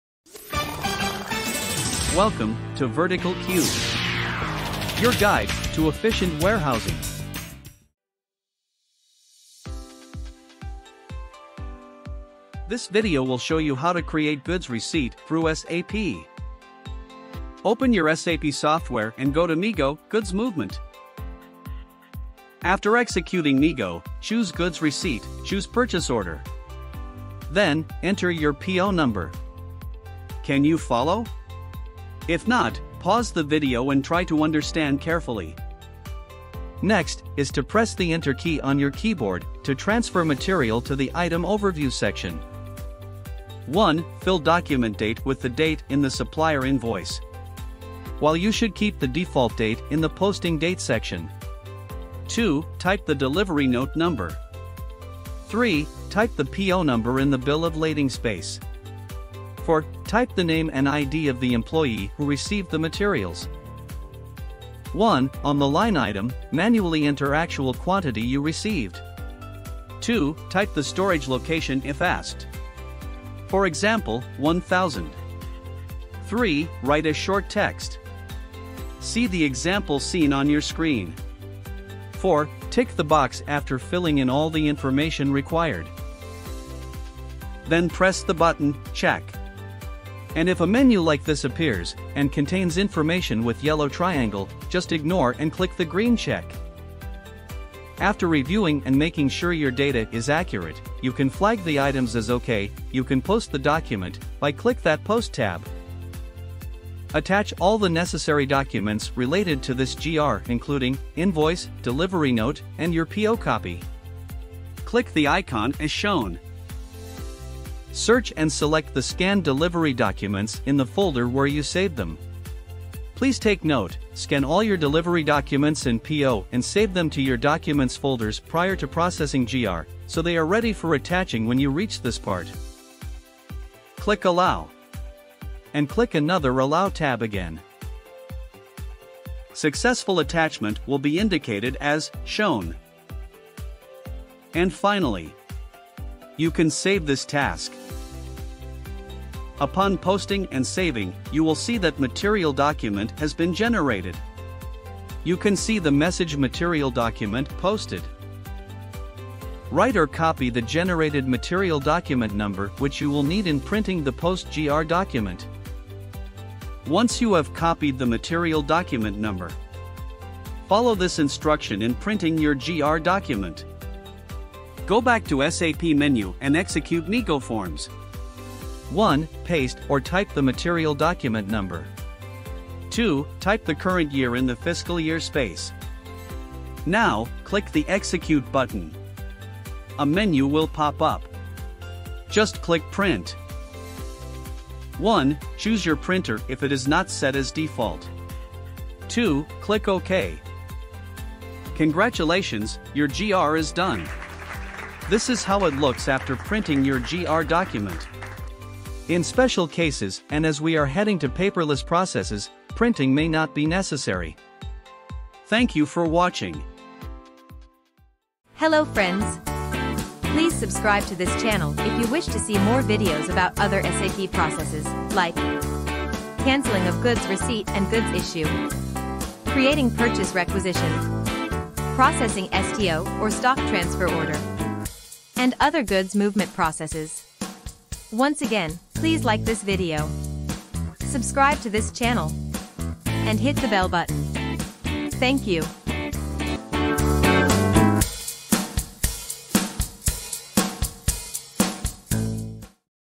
Goods Receipt with SAP MIGO Follow These Steps
Clear and easy steps in Goods Receipt (GR) against Purchase Order with SAP MIGO. After goods receipt 101, the system increases the stock of the material.
This is not for general use. Each organization may have its own distinct steps.